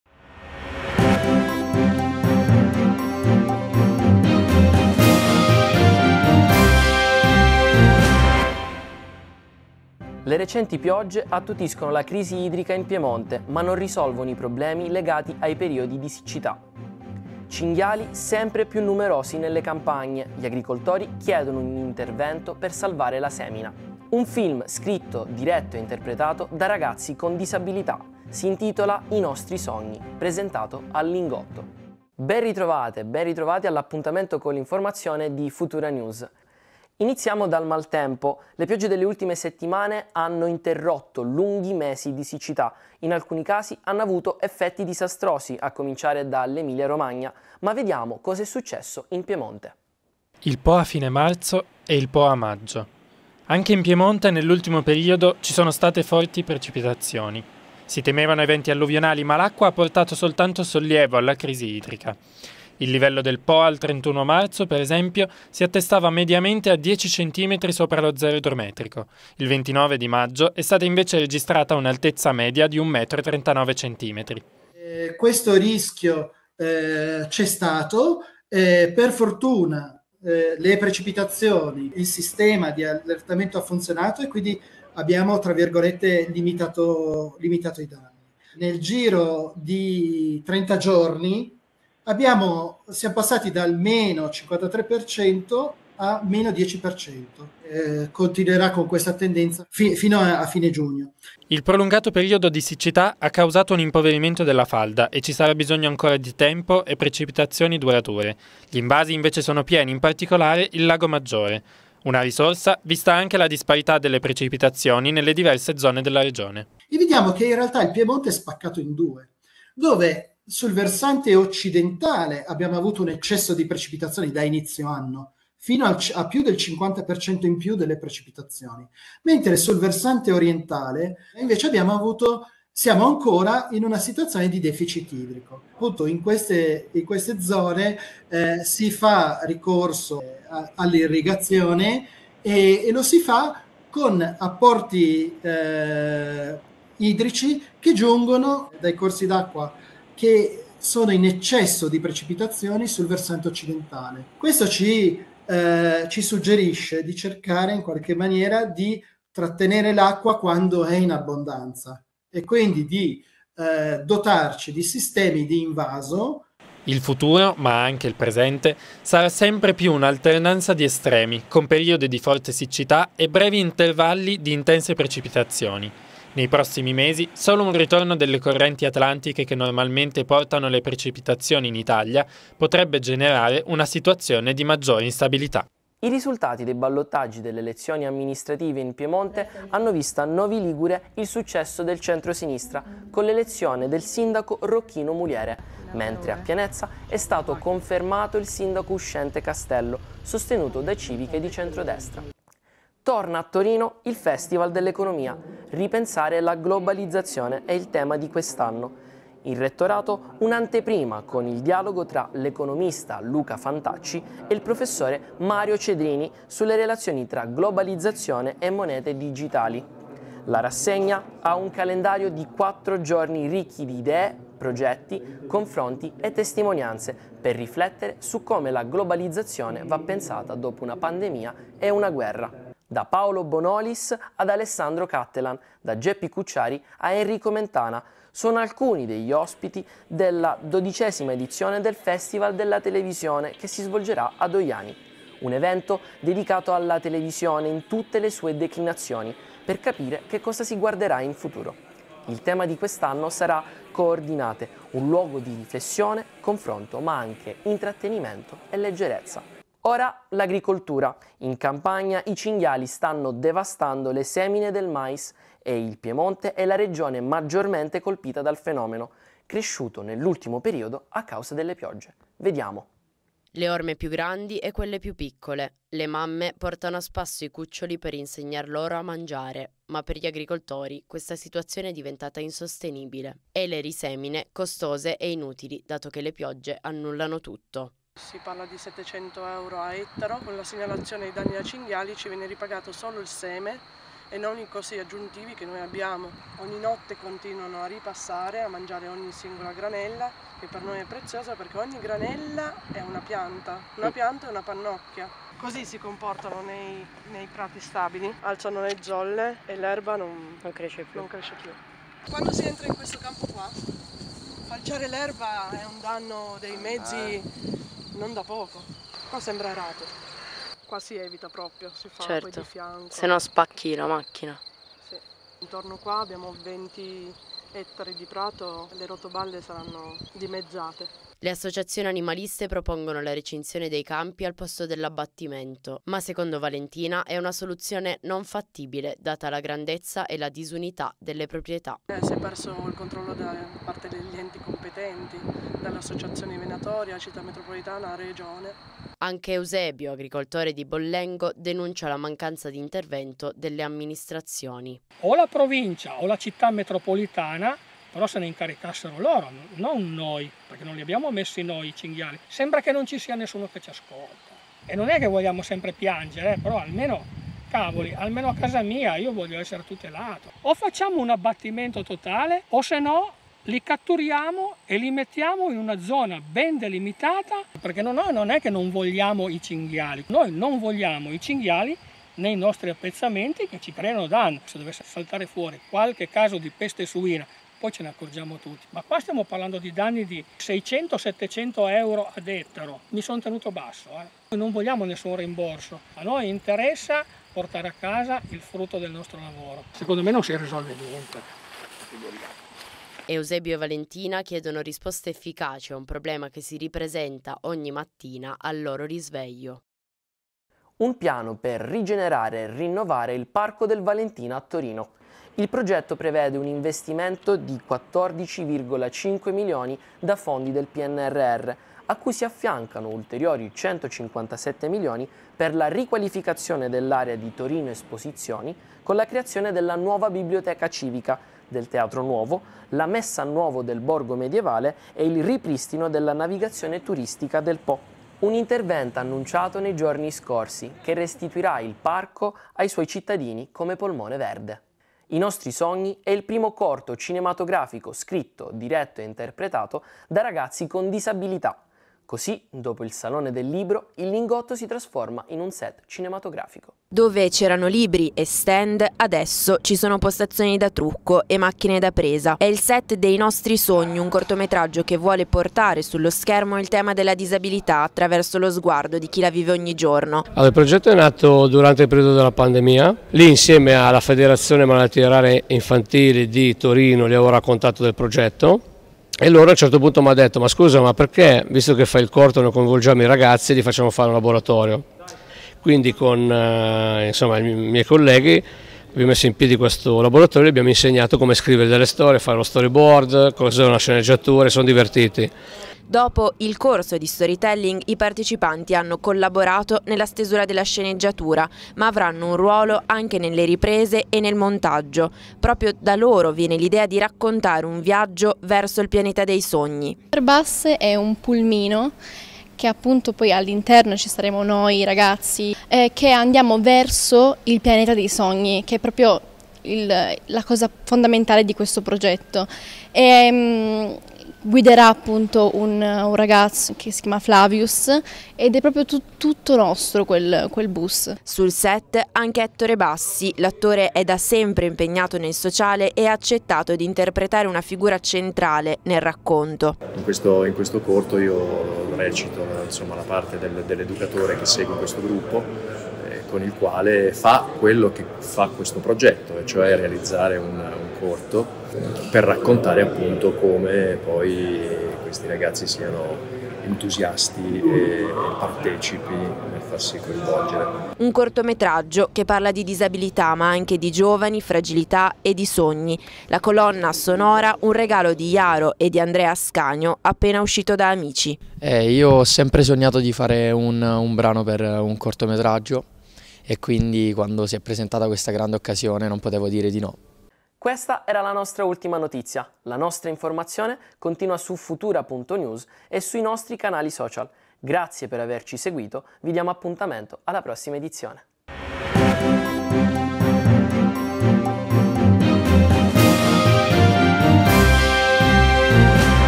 TG Futura News dell'1 giugno 2023
I servizi nell'edizione di questo TG:
Piogge e crisi idrica in Piemonte 00:50
Problema cinghiali nelle campagne 04:33
"I nostri sogni" al Lingotto 07:37